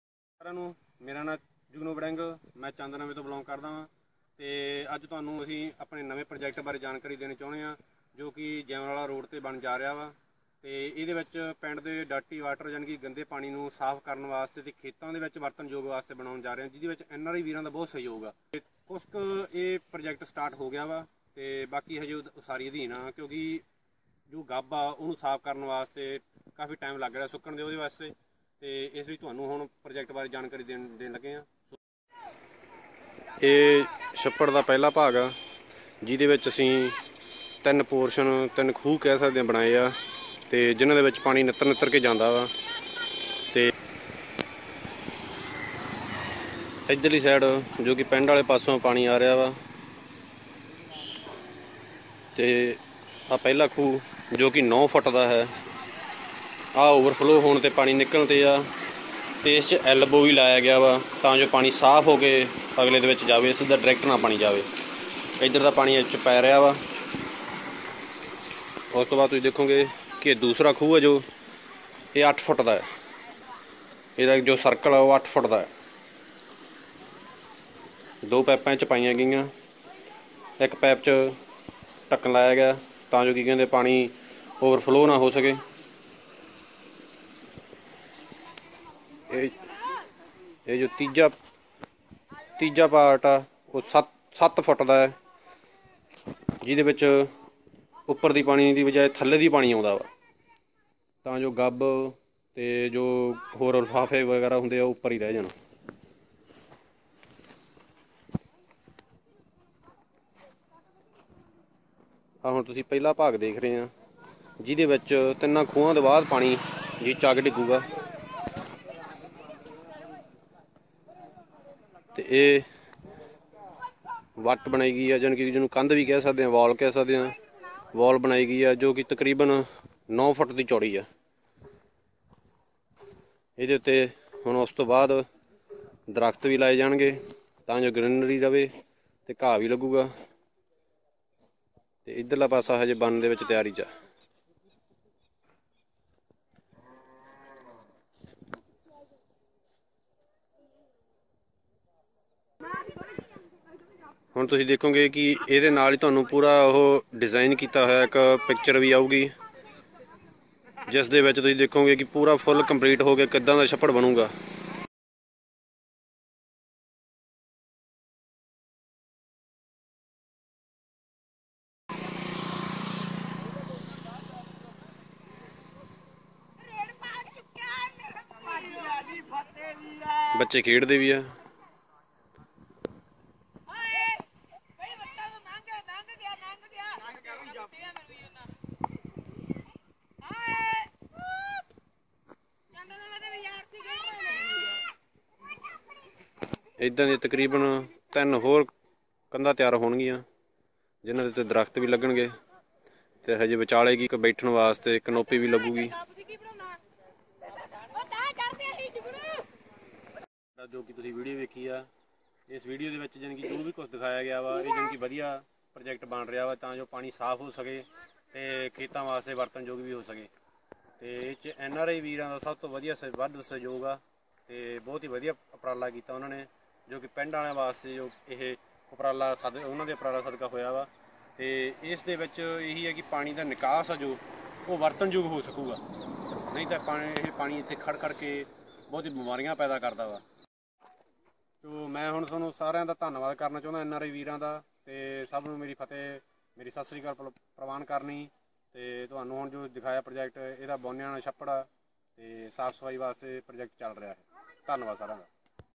Pind Chand Nawan de Boliana Shapper da Clean Water Project
pind de gande and waste pani nu saaf krke farm vich wartan yog banouna